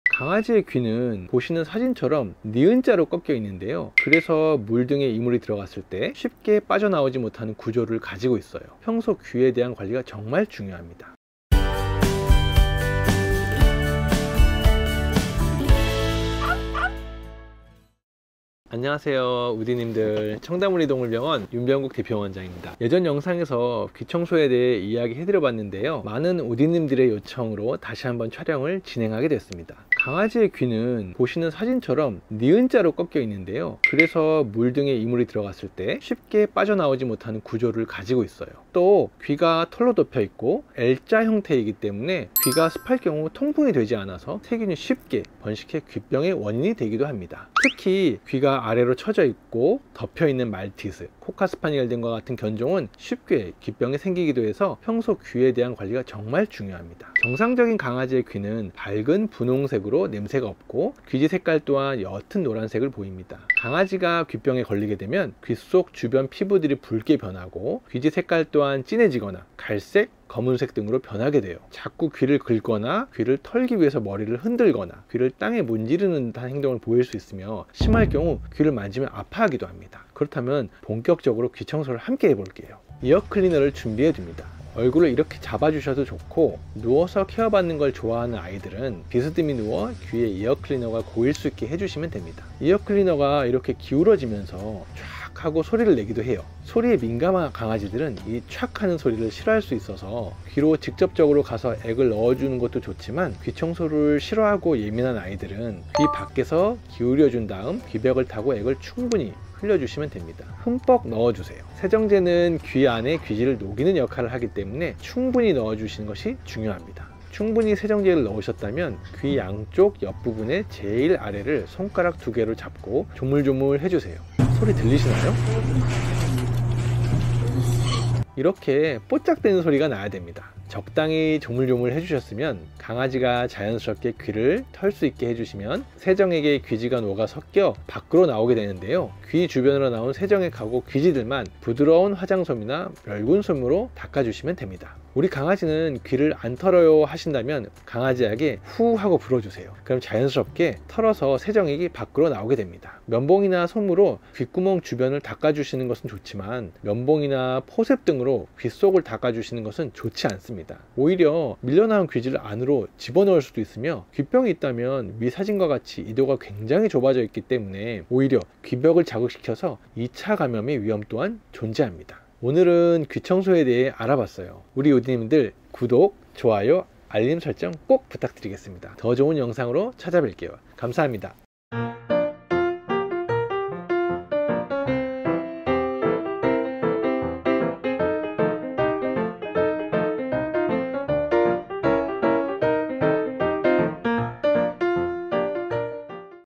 강아지 귀청소 어렵지않아요! 집에서 귀청소 쉽게하는 꿀팁 :)
함께 알아보는 강아지 귀청소!
궁금하신 점은 댓글 남겨주세요 :)